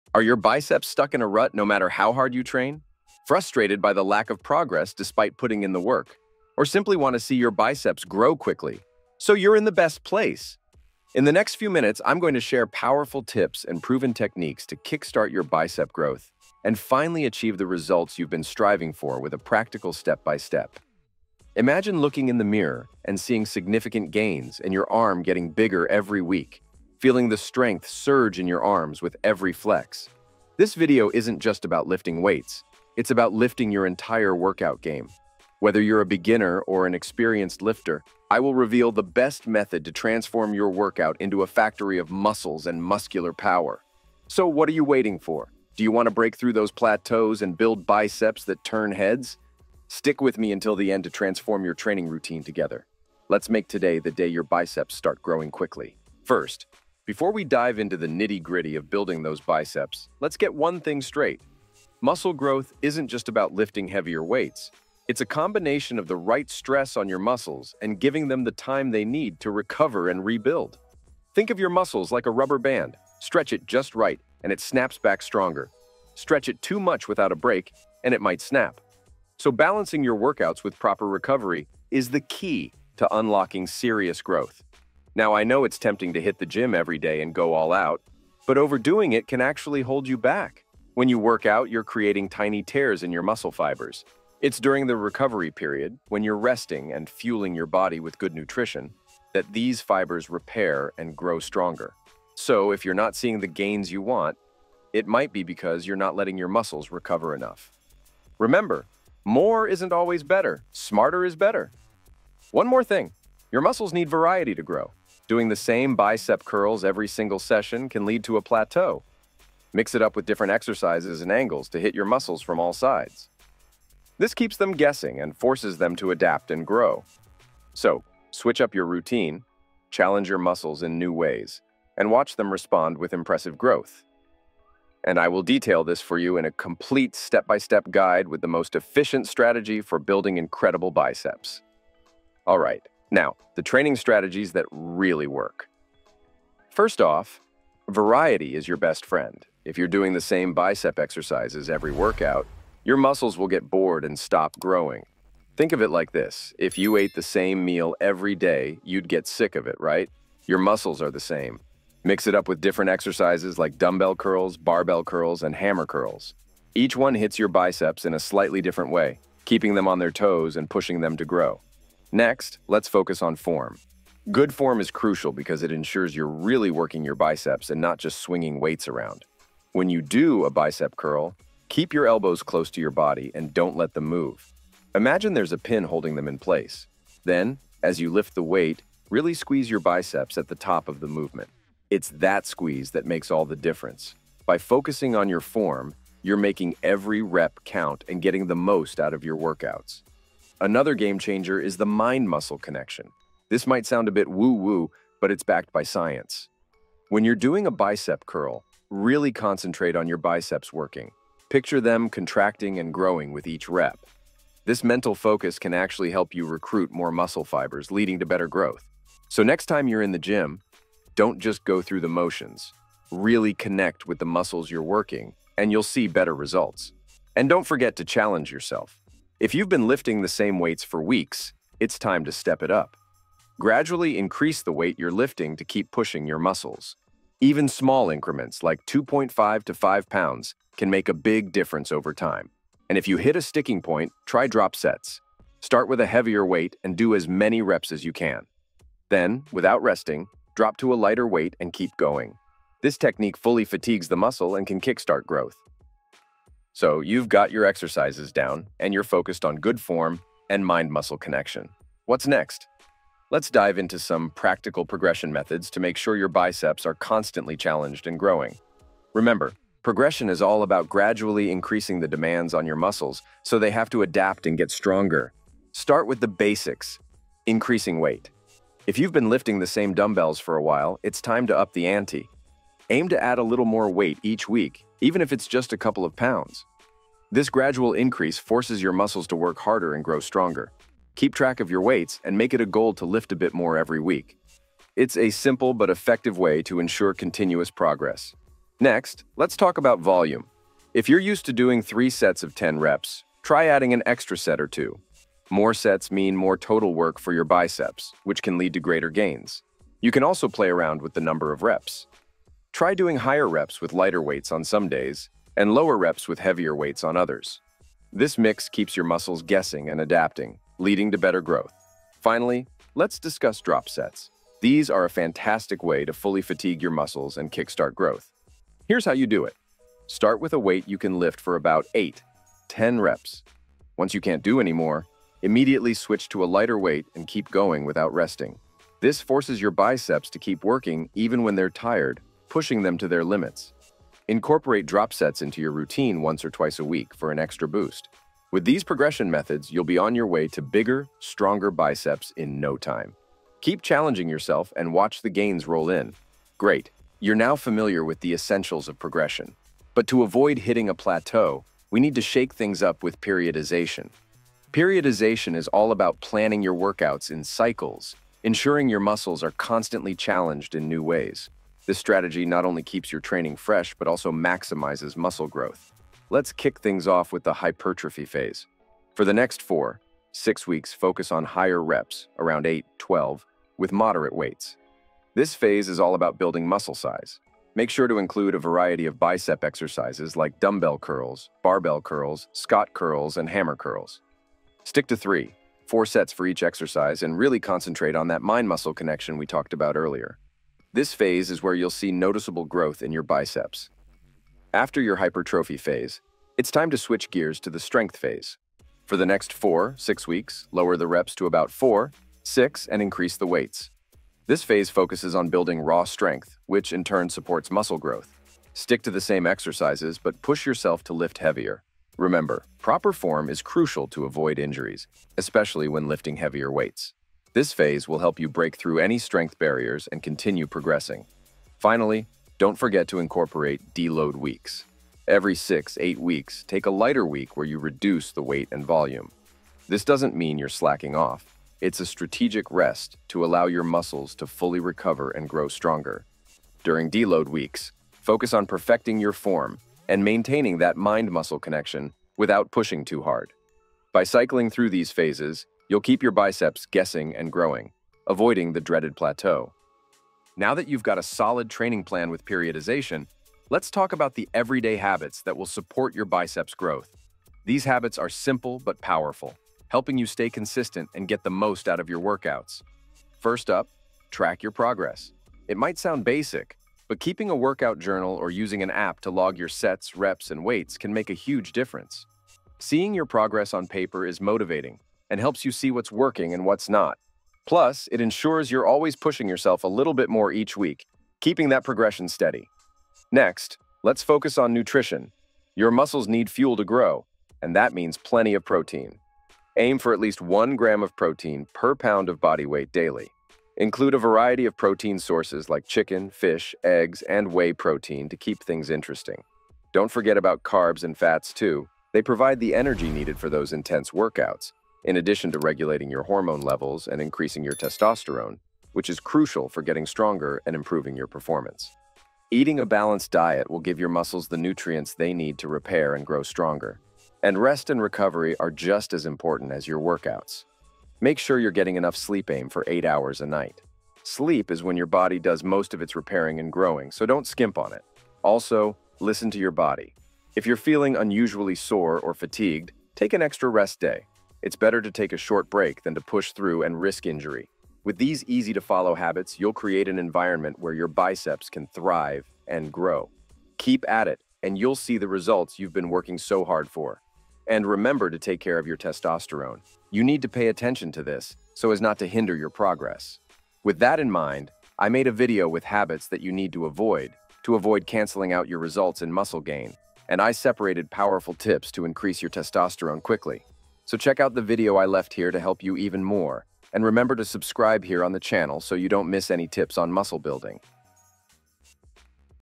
Get a Huge Biceps in 4 Weeks - Biceps Workout Guide
This is the ultimate strategies to unlock rapid arm growth. 😎
Discover the secrets behind effective training methods, smart progression techniques, and practical habits that will transform your biceps like never before. Whether you're a gym newbie or a seasoned lifter, these tips will take your arm workouts to the next level.

Get ready to:

💪   Boost your bicep size and strength faster than ever
🚀   Learn simple, powerful habits for consistent muscle gains
🚫   Avoid common mistakes that hold back your progress in the gym

🏋️‍♂️ Let's get started your journey to bigger, stronger biceps today.